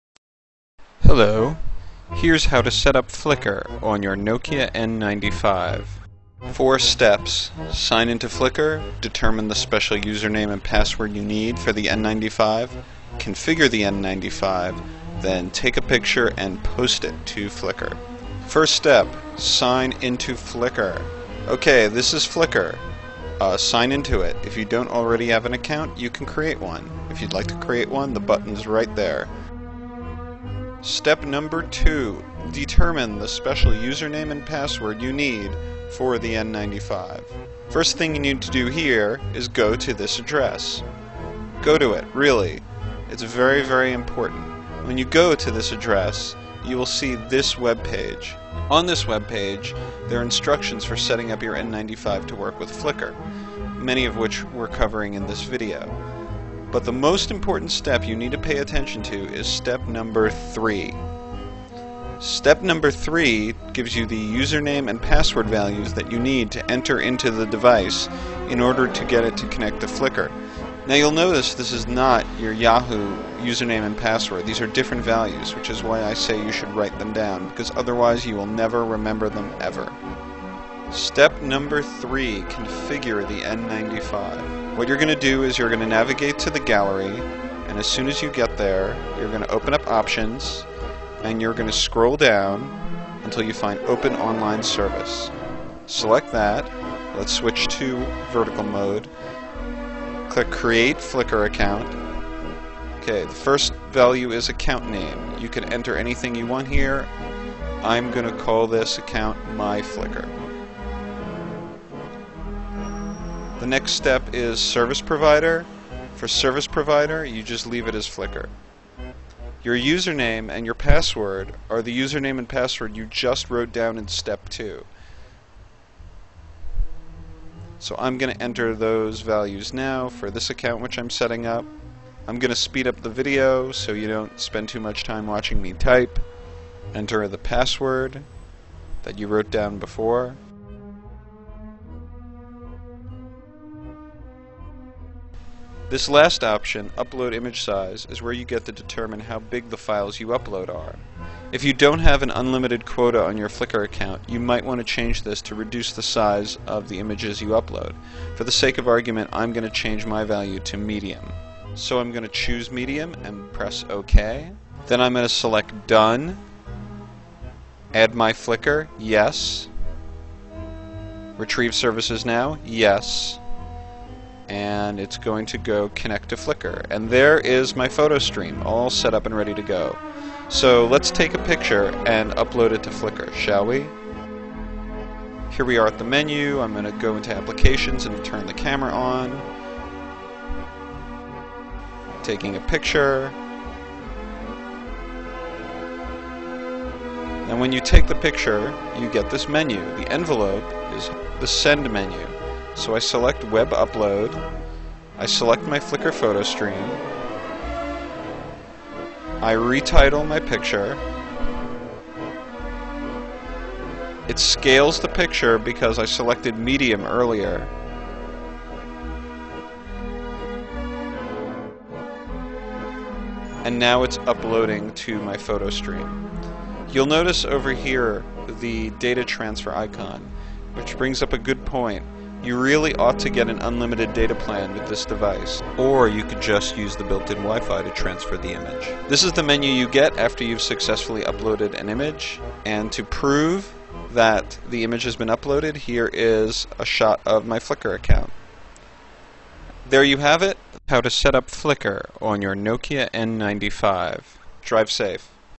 N95 Flickr Guide
How to use Flickr with the Nokia N95